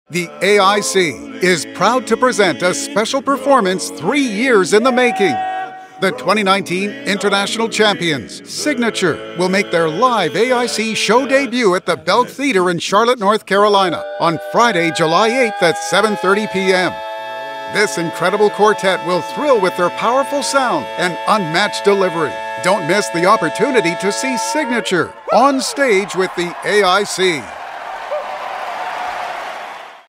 Harmony Homecoming Presents Signature!
A performance three years in making. Don't miss the opportunity to see the 2019 International Champions, Signature, perform on their first official AIC show! Tickets and Live Stream access are available at aicgold.com